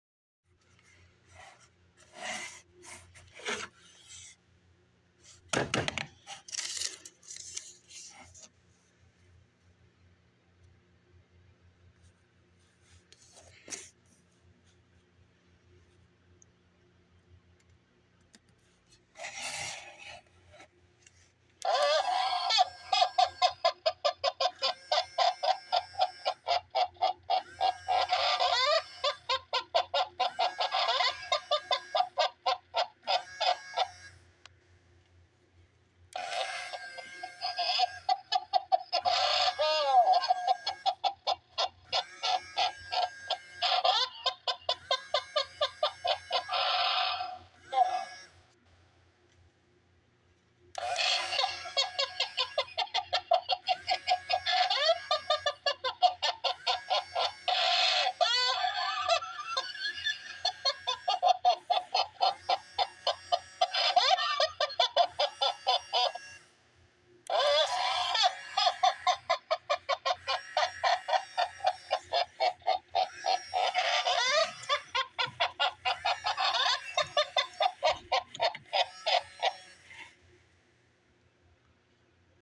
Gemmy giggling Kris Kringle
Here's my Gemmy giggling Kris Kringle I got at a flea market (yes I know it's broken)